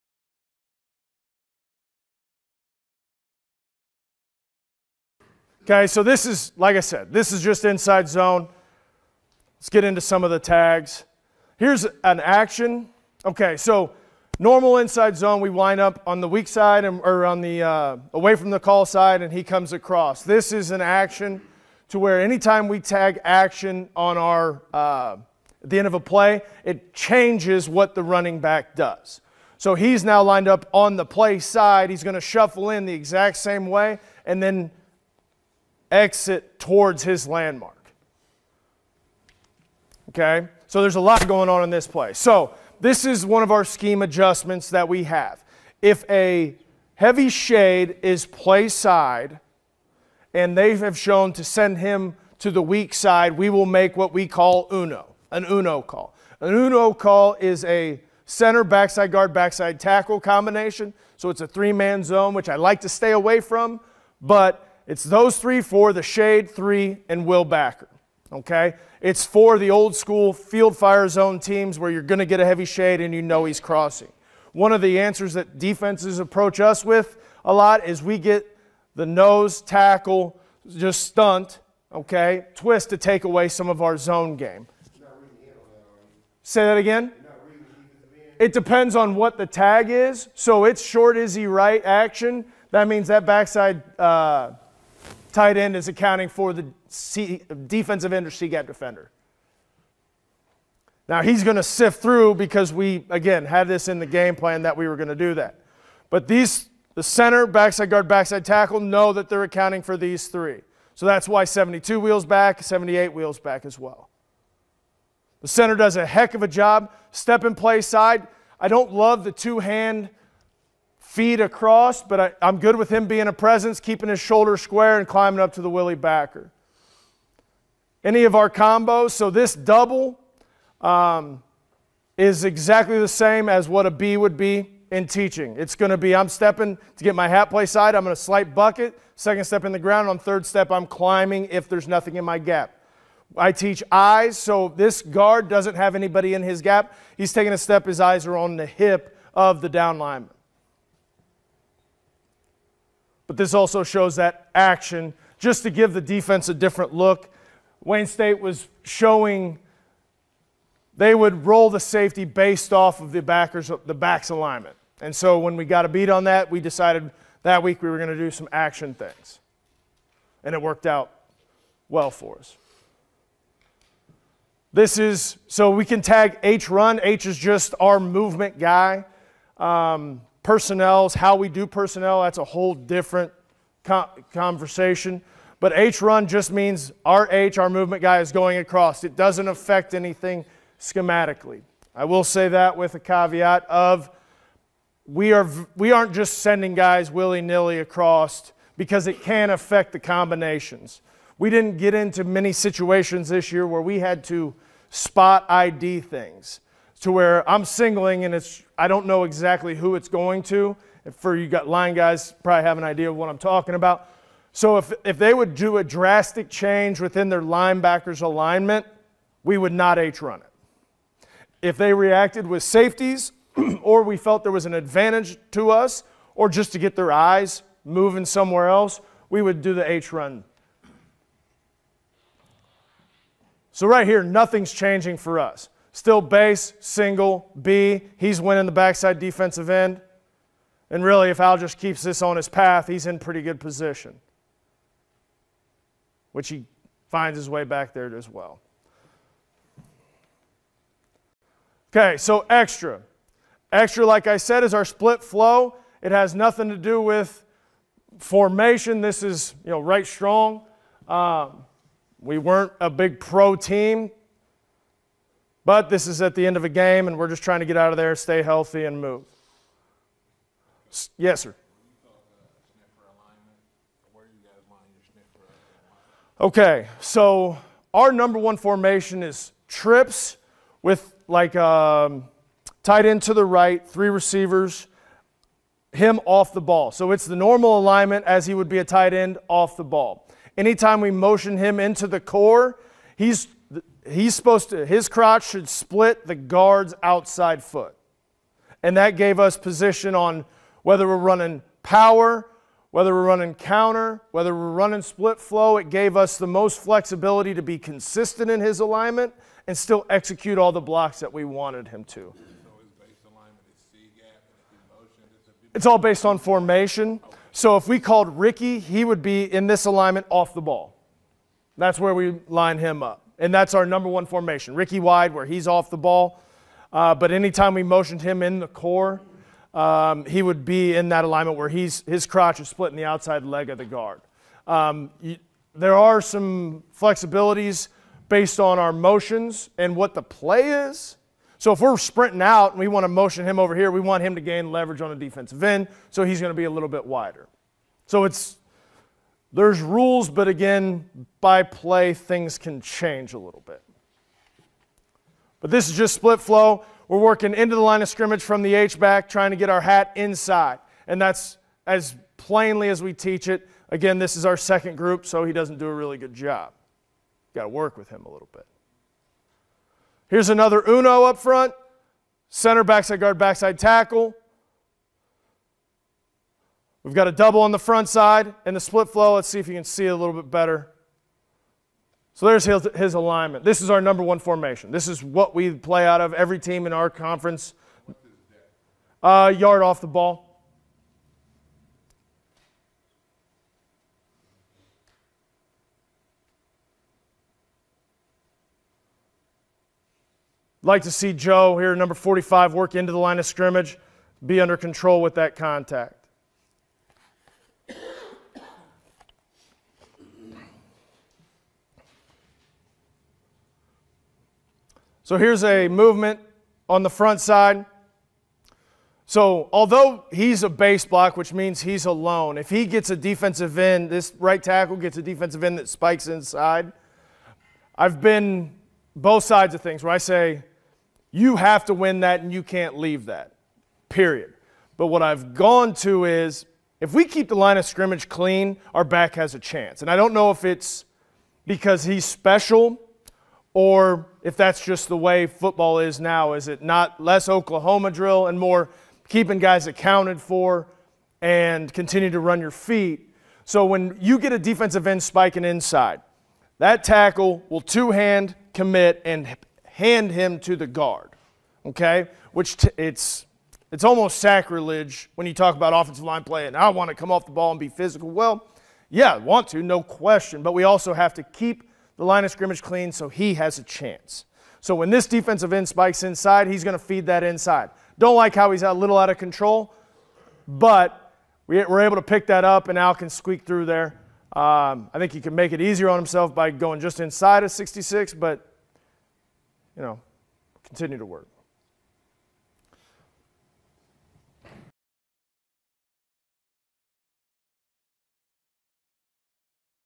Adding Tags to Inside Zone - Brad Wilson
Coach Brad Wilson, offensive line coach at the University of Indianapolis, explains a couple of their favorite ways to add tags to the inside zone scheme, and change up the blocking assignments to create an advantage for them over the offense.

Automated Transcription:

Okay, so this is, like I said, this is just inside zone. Let's get into some of the tags. Here's an action. Okay, so normal inside zone, we wind up on the weak side or on the away from the call side and he comes across this is an action to where anytime we tag action on our the end of a play, it changes what the running back does. So he's now lined up on the play side he's going to shuffle in the exact same way and then exit towards his landmark. Okay, so there's a lot going on in this place. So this is one of our scheme adjustments that we have. If a heavy shade is placed side and they've shown to send him to the weak side we will make what we call uno uno uno call Ludo call is a center backside guard backside tackle combination. So it's a three man zone which I like to stay away from, but it's those three for the shade three and we'll back. Okay, it's for the old school field fire zone teams where you're going to get a heavy shade and you know he's crossing. One of the answers that defenses approach us with a lot is we get the nose tackle, just stunt. Okay twist to take away some of our zone game. Say that again. It depends on what the tag is. So it's short Is he right action. That means that backside tight end as accounting for the sea defensive industry get defender. Now he's going to sift through because we again have this in the game plan that we were going to do that. But these center backside guard backside tackle know that they're accounting for these three. So that's why 72 wheels back 78 wheels back as well. The center does a heck of a job, step in place side. I don't love the two hand, feet across, but I'm good with him being a presence keeping a shoulder square and climbing up to the willie backer any of our combos. So this double is exactly the same as what a B would be in teaching. It's going to be I'm stepping to get my hat play side I'm going to slight bucket second step in the ground on third step I'm climbing if there's nothing in my gap, I teach eyes so this guard doesn't have anybody in his gap. He's taking a step his eyes are on the hip of the downline. But there's also shows that action just to give the defense a different look. Wayne State was showing they would roll the safety based off of the backers of the backs alignment. And so when we got to beat on that we decided that we were going to do some action things. And it worked out well for us. This is so we can tag H Ron H is just our movement guy personnels how we do personnel that's a whole different conversation. But H run just means our HR movement guy is going across it doesn't affect anything. Schematically, I will say that with a caveat of We Are we aren't just sending guys willy nilly across because it can affect the combinations. We didn't get into many situations this year where we had to spot ID things to where I'm singling and it's I don't know exactly who it's going to it for you got line guys probably have an idea of what I'm talking about. So if they would do a drastic change within their linebackers alignment, we would not Atran it if they reacted with safeties or we felt there was an advantage to us or just to get their eyes moving somewhere else we would do the H run so right here nothing's changing for us. Still bass single b He's winning the backside defensive end and really if I'll just keep this on his path he's in pretty good position which he finds his way back there as well. Okay, so extra extra like I said is our split flow it has nothing to do with formation this is you know, right strong we weren't a big pro team but this is at the end of the game and we're just trying to get out of there stay healthy and move

Yes sir. Okay, so our number one formation is trips with like, tight into the right three receivers him off the ball so it's the normal alignment as he would be a tight end off the ball. Anytime we motioned him into the core. He's, he's supposed to his crotch should split the guards outside foot and that gave us position On, whether we're running power, whether we're running counter, whether we're running split flow, it gave us the most flexibility to be consistent in his alignment, and still execute all the blocks that we wanted him to. It's all based on formation. So if we called Ricky, he would be in this alignment off the ball. That's where we lined him up. And that's our number one formation Ricky wide where he's off the ball. But anytime we motioned him in the core, he would be in that alignment where he's his crotch is split in the outside leg of the guard.